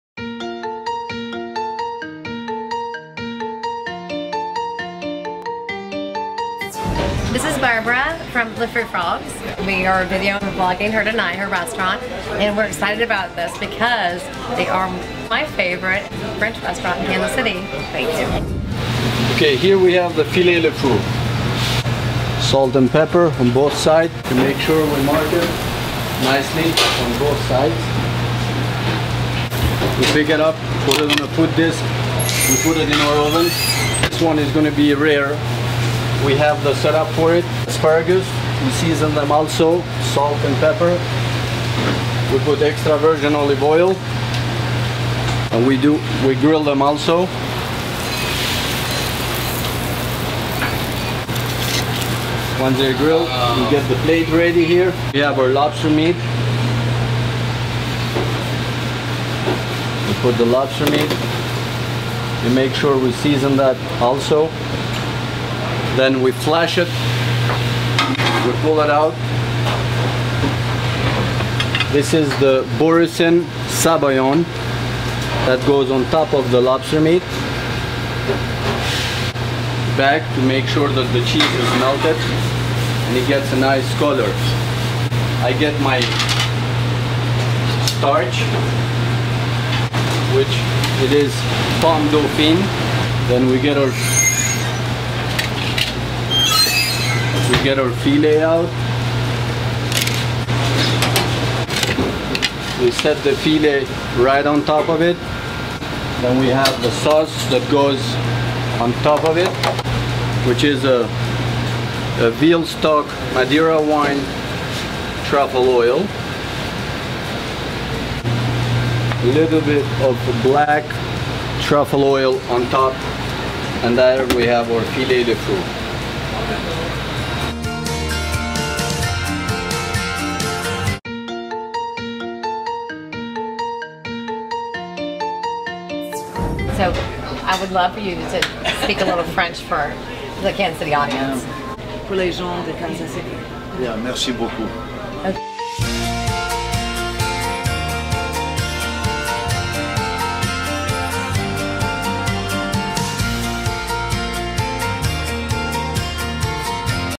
Le Fou Frog French bistro | KC Linked behind the kitchen (delicious destinations)
What a fun experience this was behind Le Fou Frogs kitchen.  Enjoy, this amazing chef at work!  He is unbelievably talented.